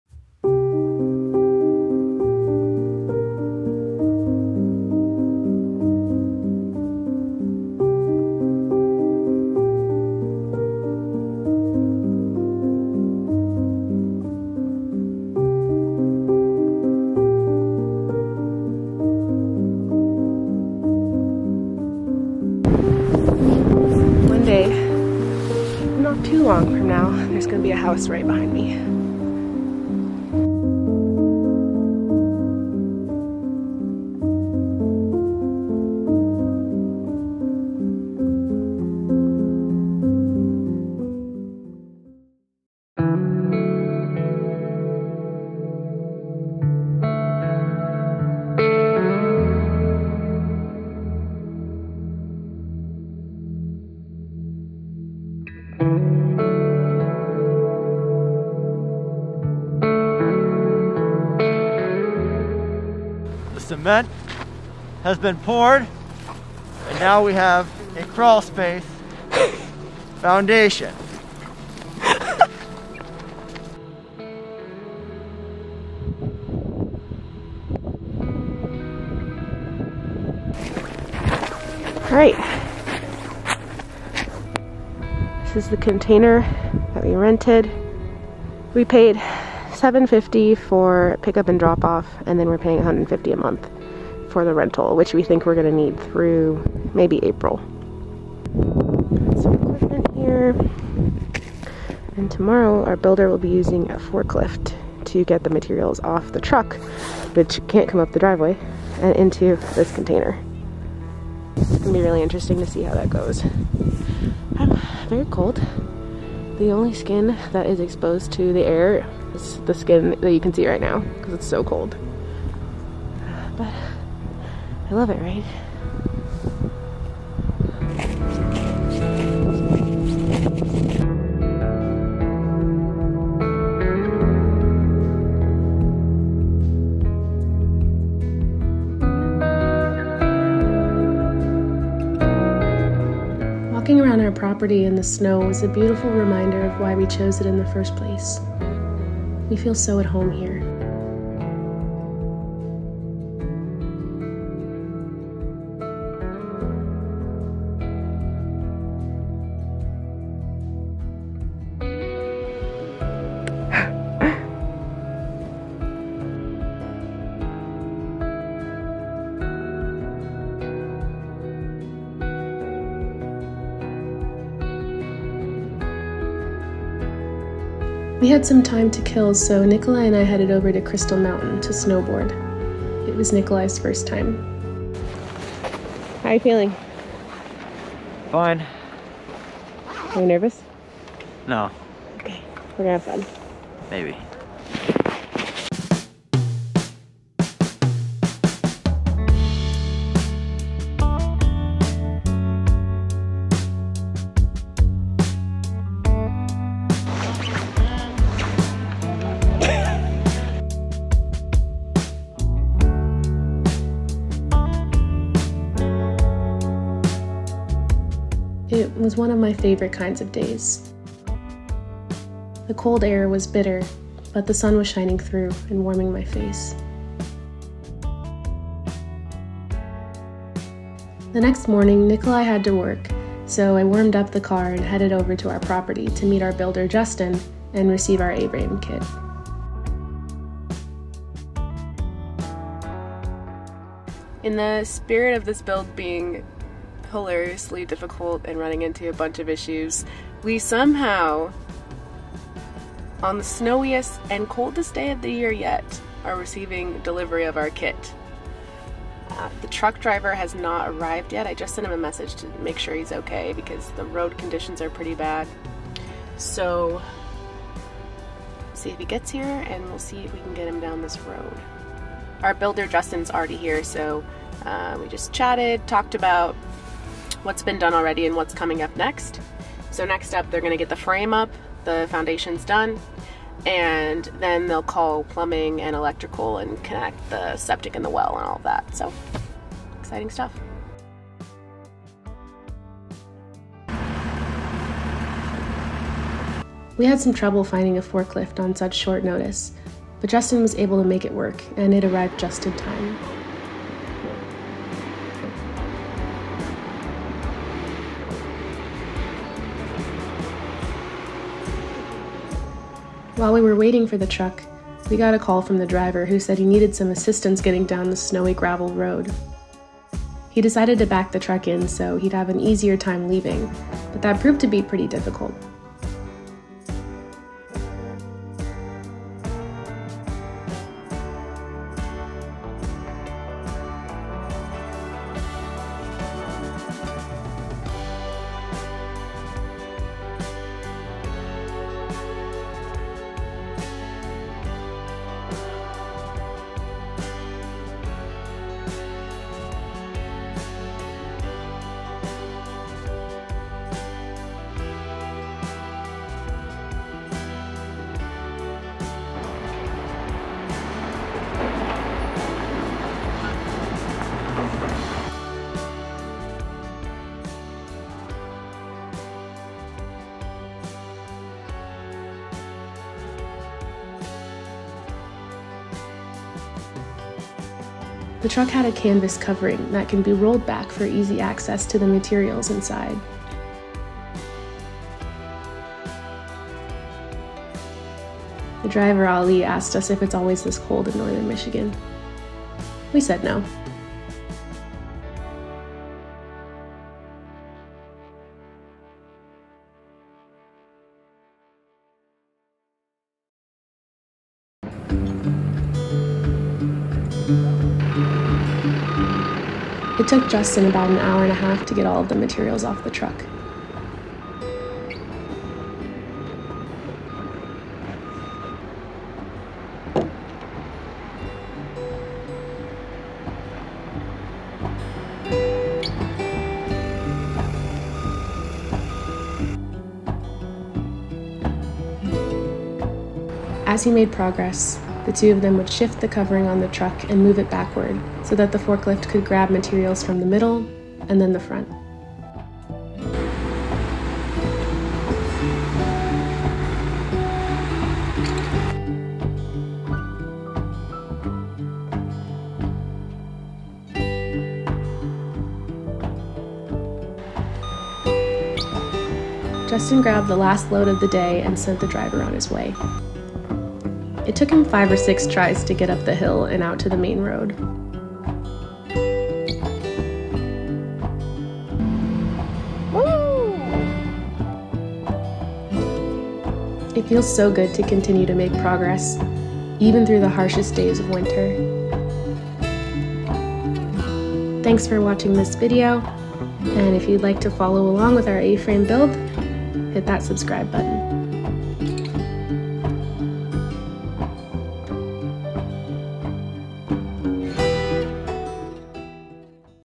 Things are finally starting to look up
Hi friends :) Our Avrame Trio 120 Structural kit was finally delivered! It was a very snowy and cold day in Northern Michigan, but we got it done and we've got what we need to get started with building the structure. Leave any questions about the process below!